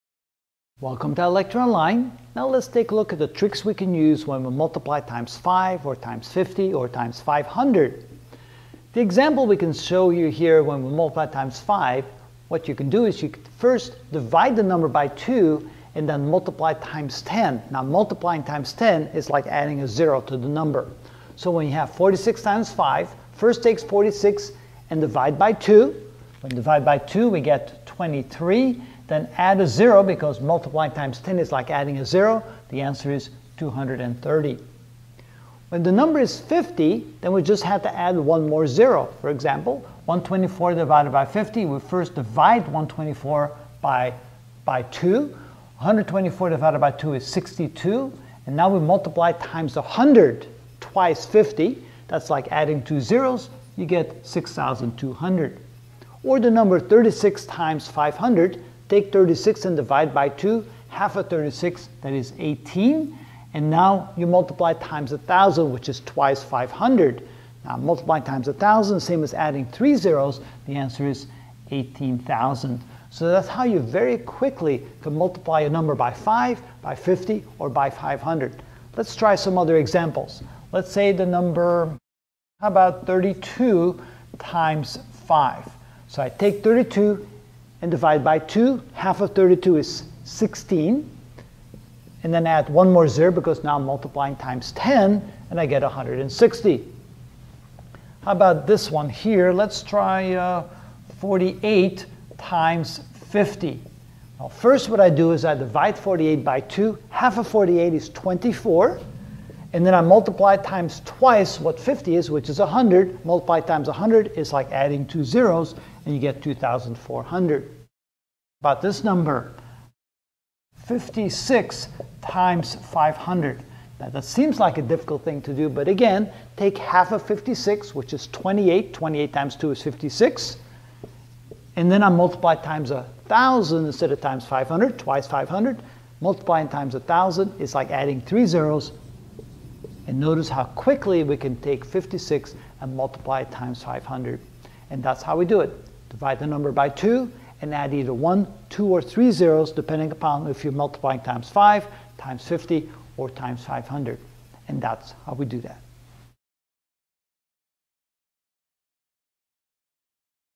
24.Math What Every 5th Grader Should Know (24 of 25) Ch 3 How to Multiply Any Number by 5, 50, 500
How to Multiply Any Number by 5, 50, 500 | By Predestined Future Global Leaders Academy